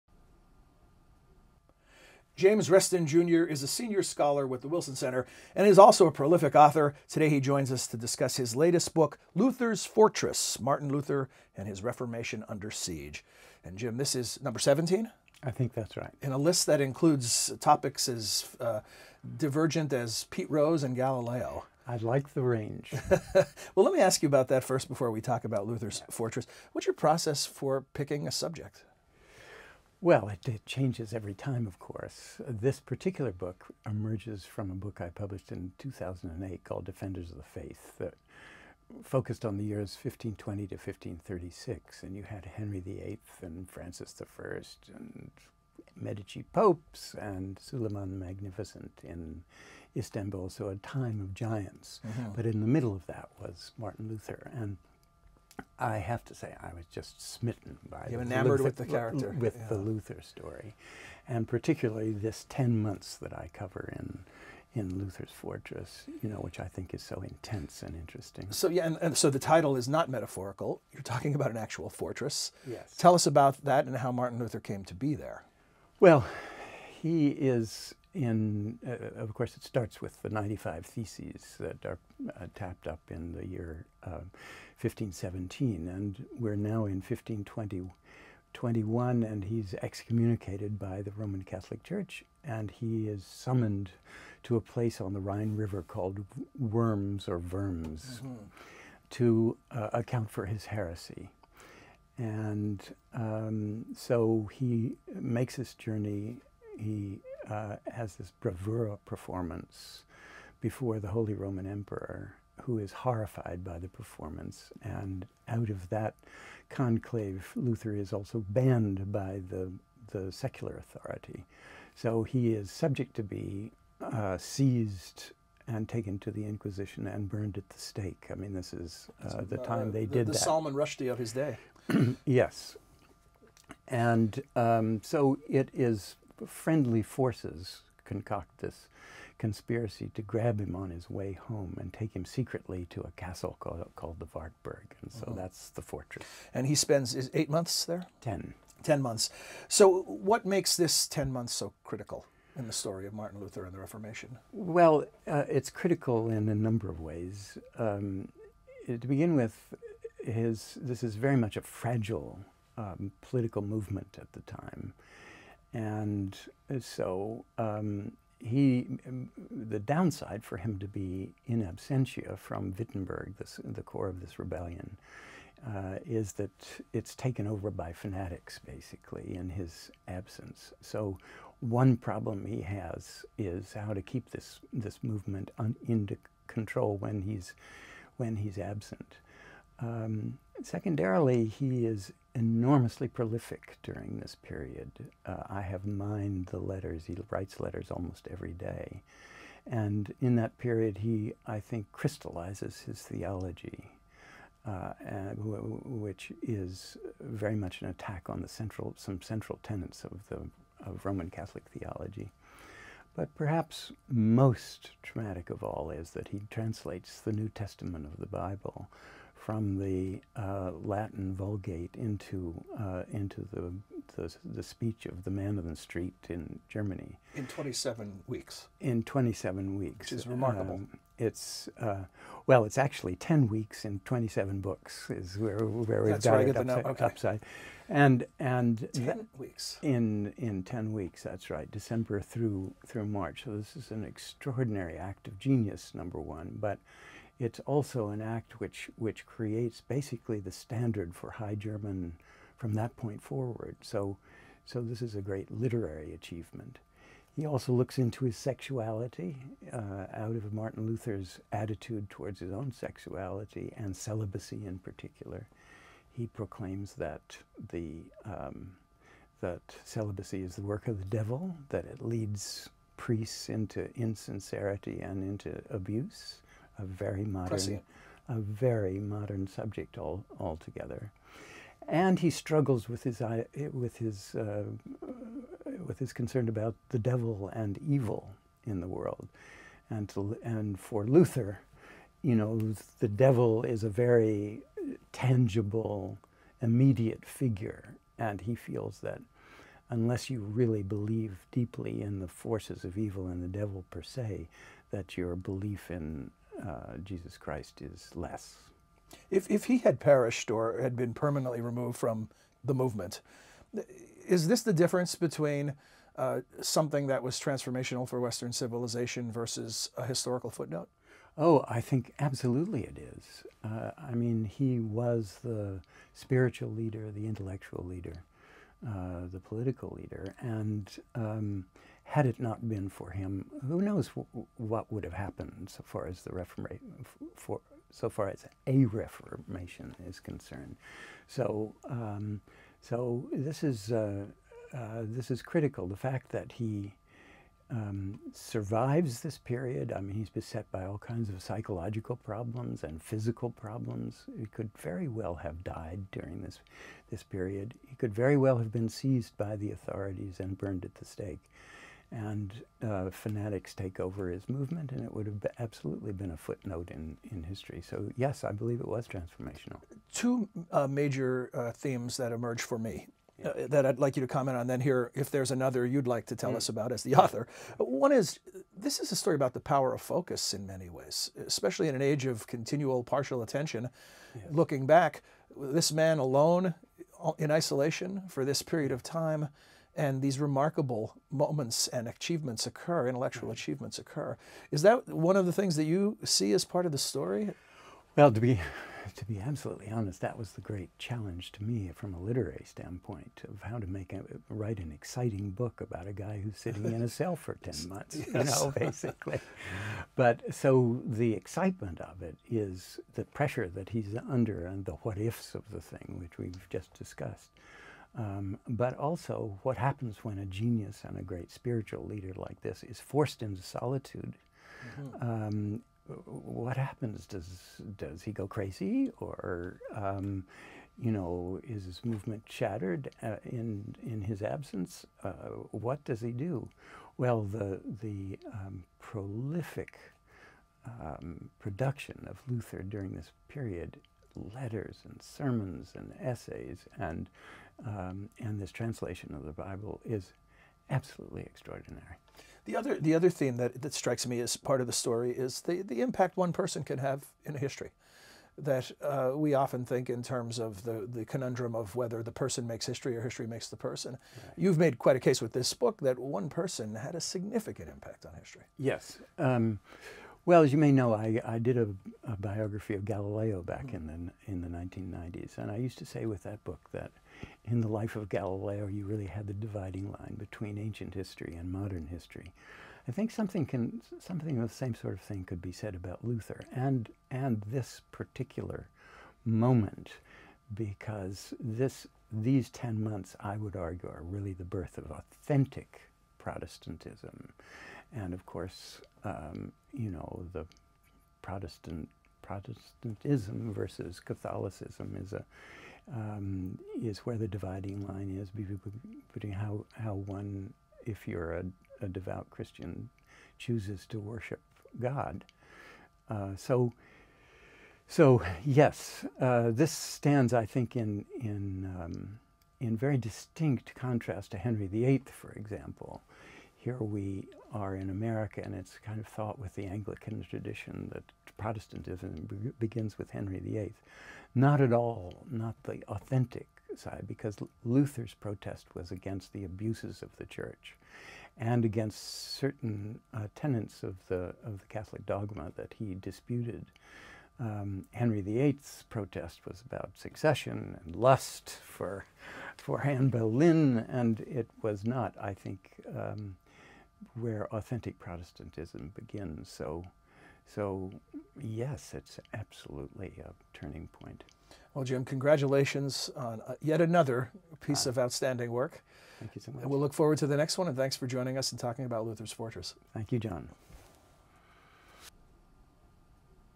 Luther’s Fortress: Martin Luther and His Reformation Under Siege
Prolific author, acclaimed historian, and Wilson Center Senior Scholar, James Reston’s latest book draws on primary source correspondence, notes, and other writings to provide a gripping portrait of what may be the most productive months of Martin Luther’s life. Luther’s Fortress: Martin Luther and His Reformation Under Siege, chronicles a critical but little-known episode in Luther’s life, revealing its pivotal role in the history of Christianity. That’s the focus of this edition of  Wilson Center NOW.